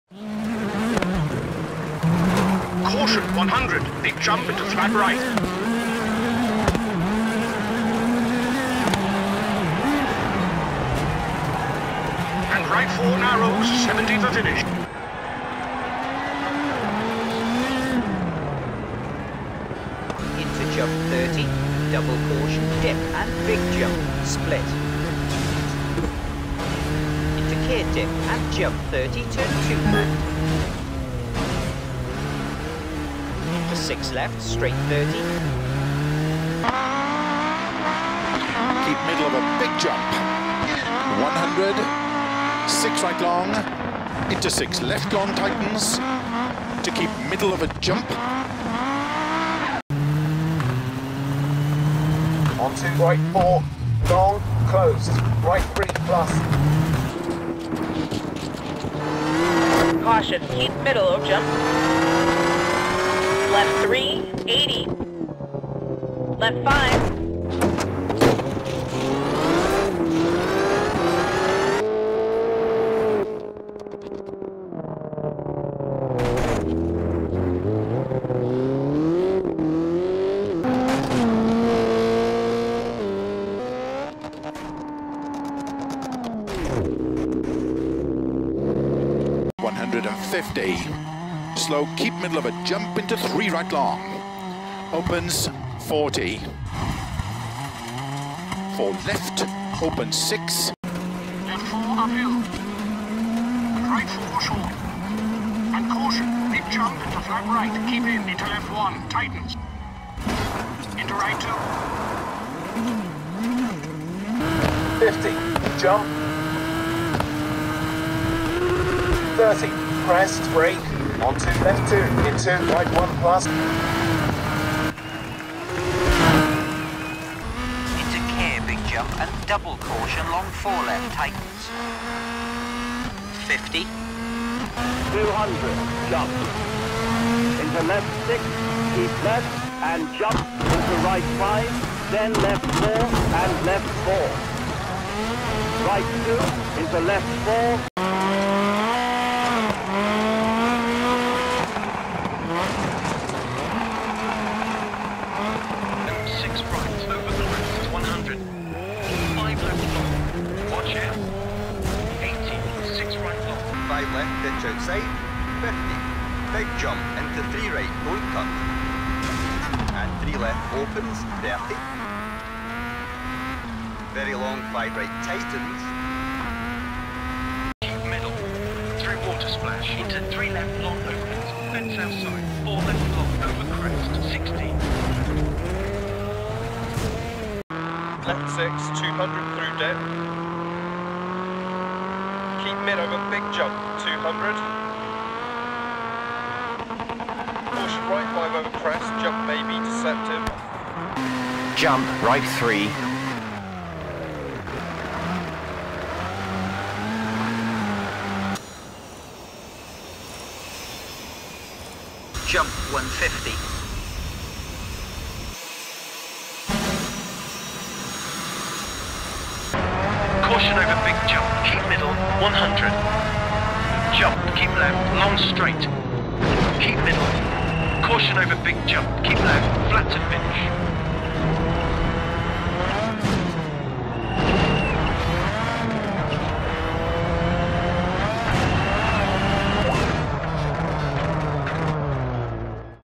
Jumps in random rally games
I like jumping, so I decided to collect a selection of my previous videos